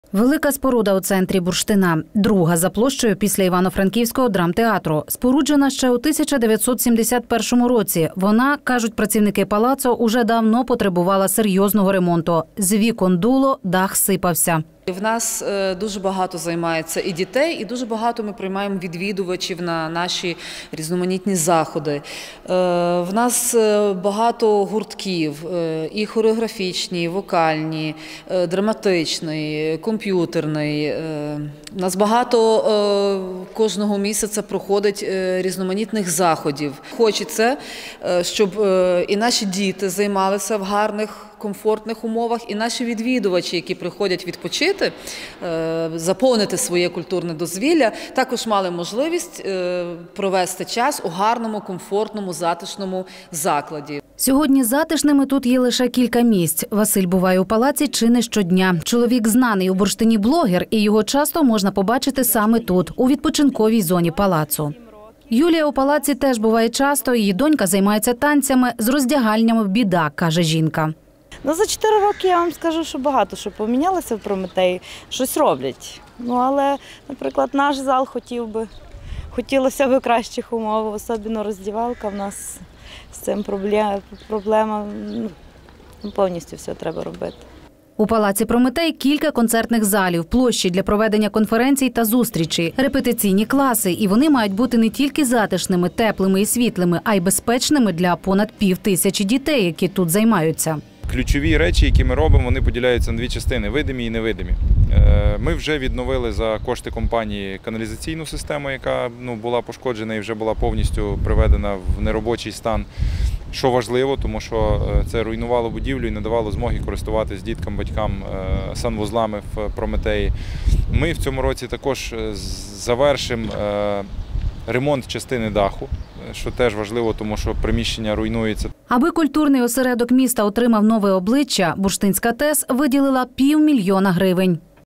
Оновити культурно-мистецький простір взялися в Бурштині
Оновити культурно-мистецький простір взялися в Бурштині. У палаці культури «Прометей» замінили вікна, відремонтували дах та взялися за оновлення бібліотеки та студії звукозапису.
Організація неприбуткова, шукали ґранти і зверталася до благодійників. Гроші знайшли - оновлюють палац силами і коштами Бурштинської ТЕС.